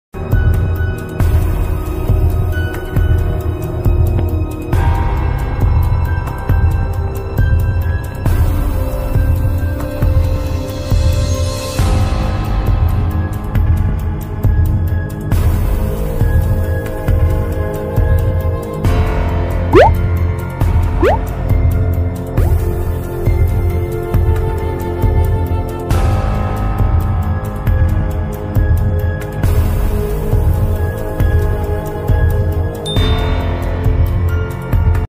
Are there dancing fish in the world ?
What kind of dancing fish ok🐠is this in this world is it ?
3d animation video on google maps and google earth view 😱

I was surprised to see such a big dancing fish 🐟 in the world🌎. on google earth and google maps view 😱. 3D animation video.

find out world biggest fish dance🐟 .
google maps view in find out funny dance poss fish🐟.